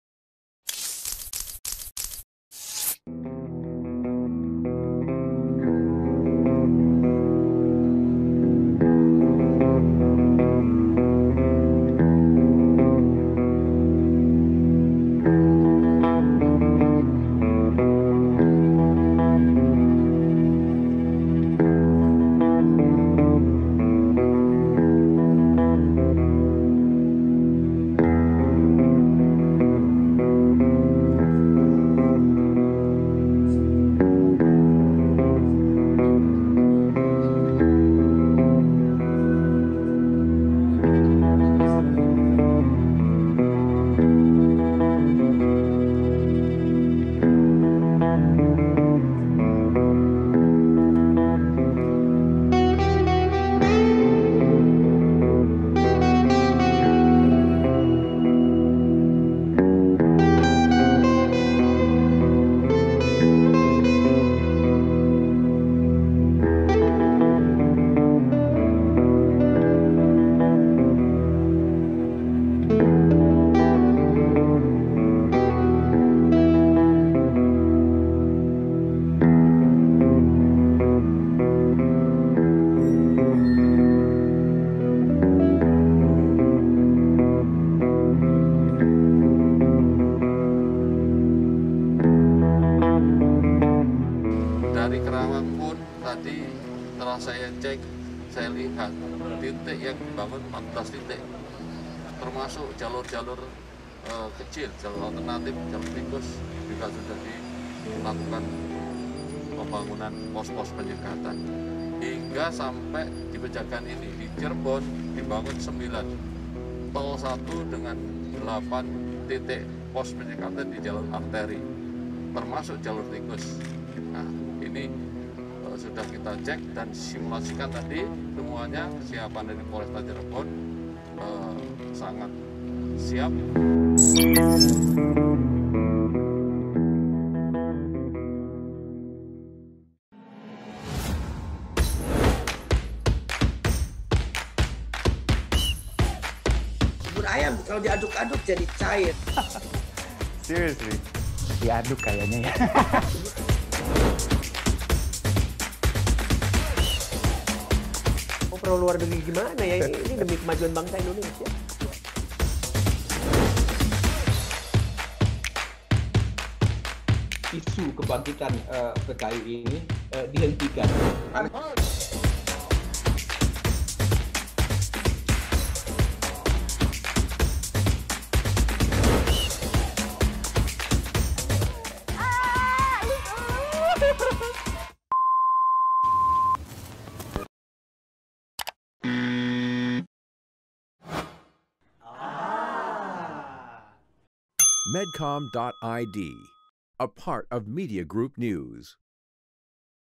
Kakorlantas Polri Cek Kesiapan Penyekatan Mudik Lebaran 2021
Kepala Korlantas Polri Irjen Pol Istiono mengecek kesiapan titik-titik penyekatan untuk mencegah masyarakat mudik lebaran 2021 sesuai keputusan pemerintah tentang peniadaan mudik. Kakorlantas Polri pastikan seluruh jalur siap dijaga ketat untuk mencegah pemudik.

Titik penyekatan awal jalur yang mengarah ke Jawa Tengah dan Jawa Barat yaitu di kilometer 31 tol Jakarta Cikampek. Di sini, Kakorlantas Polri Irjen Pol Istiono mendapat pemaparan dari jajaran Dirlantas Polda Metro Jaya terkait skema penyekatan peniadaan mudik.

Selanjutnya di Jalur Arteri yaitu di pos tangguh Karawang, Kakorlantas juga mendapatkan penjelasan pos penyekatan mencegah pemudik. Kakorlantas mengaku puas dengan kesiapan anggota menghalau pemudik untuk diputarbalikkan.

Operasi ketupat tahun 2021 ini  merupakan operasi kemanusiaan dengan tindakan persuasif dan humanis yaitu dengan memutarbalikkan arah. Tujuannya adalah untuk memutus rantai penyebaran COVID-19.